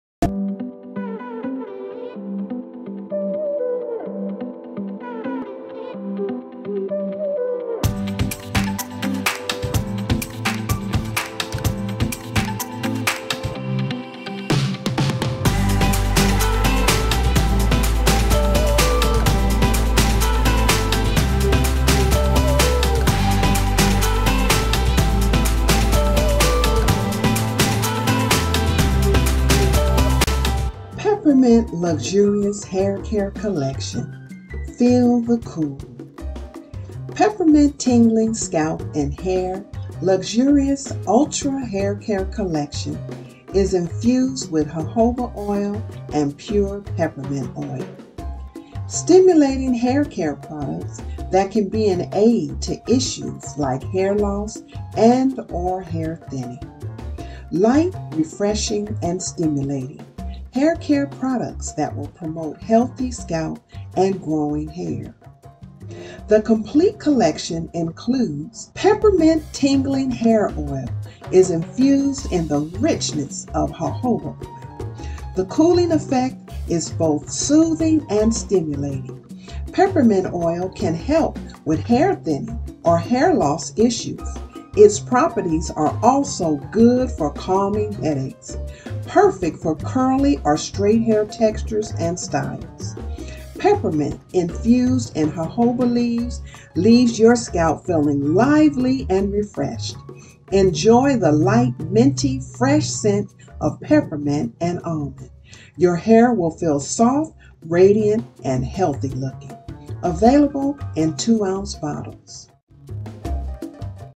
PEPPERMINT OIL for those who wear SISTERLOCKS | BeautifulHairProducts.com | Rene Michelle Floyd
Did you know that it is okay to moisturize your SISTERLOCKS?

UPDATE!

*If you found any value in this video, give it a 'thumbs up.'
*Add it to a playlist if you want to view it later.
lllllllllllllllllllllllllllllllllllllllllllllllllllllllllllllllllllllll